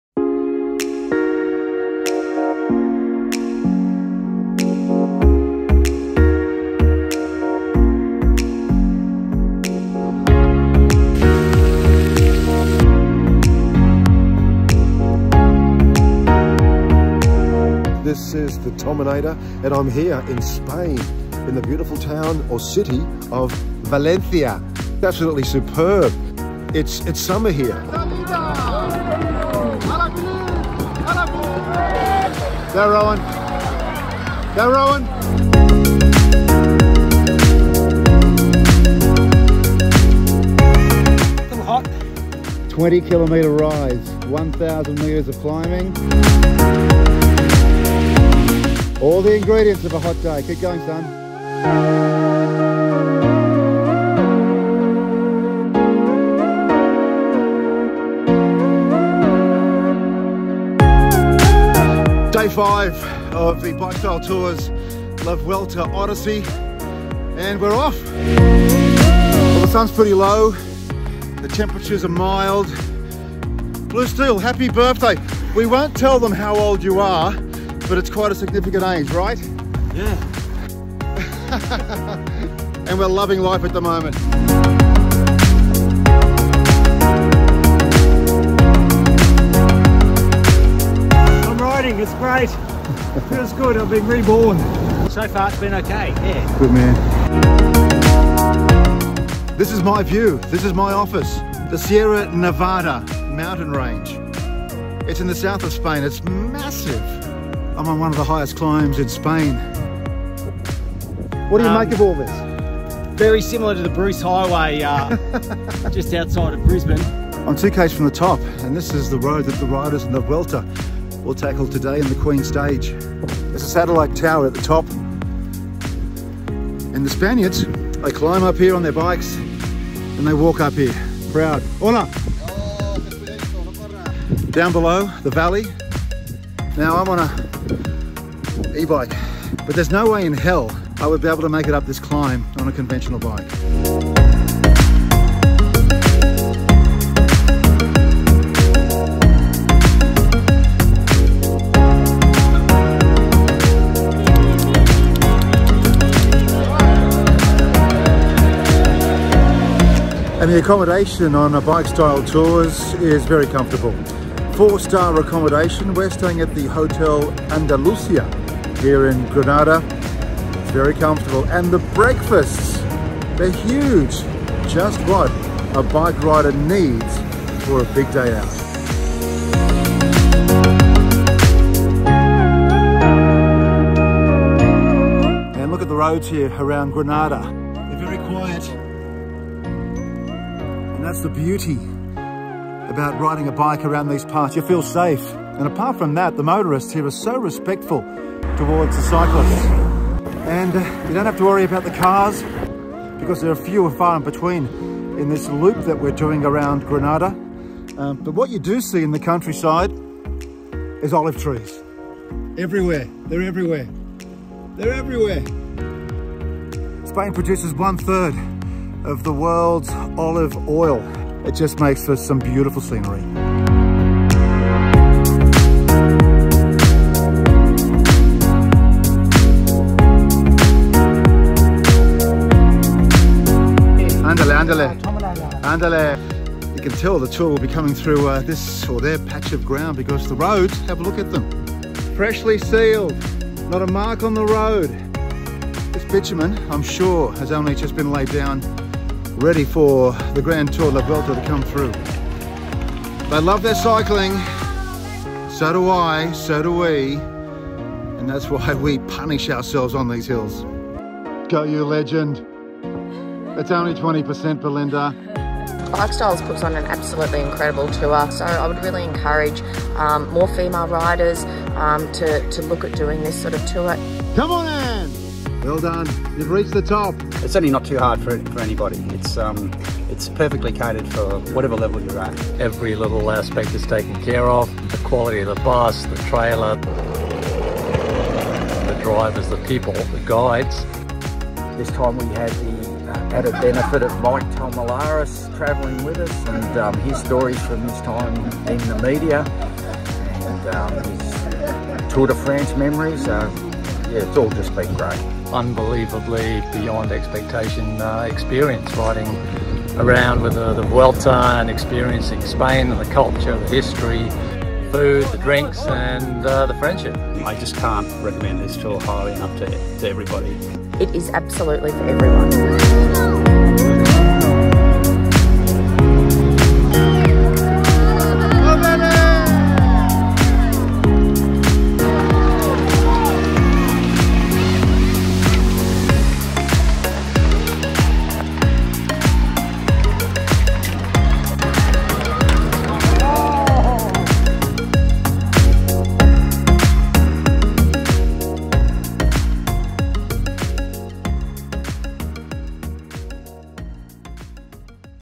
Cycling in Spain with Bikestyle Tours La Vuelta
From the Mediterranean coast to the high mountains of the notorious Sierra Nevada, Bikestyle Tours took guests on a cycling holiday like no other.

The scenery, the culture, the delights that Spain has to offer and the professional staff made it a two week journey to remember.